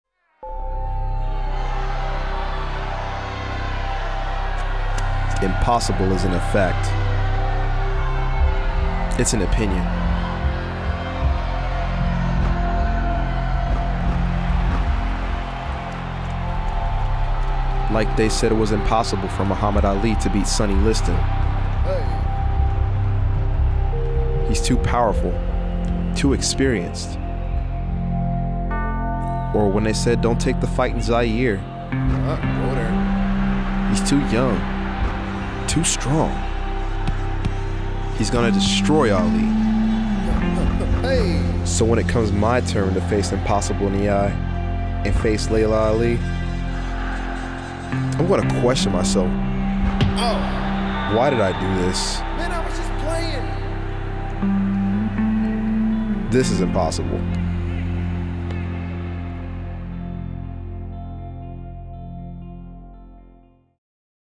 Adidas Ali-Snipez Parody Remake
Sound for Interactive Media Project-

Decided to have some fun in just doing this commercial over with the help of my friend Touch Blevins. THIS IS ALL IN FUN AS WELL AS A GRADE! LOL. I AM A BIG FAN OF THE ALI'S, NOR AM I AGAINST ANY ADIDAS PRODUCTS WHATSOEVER!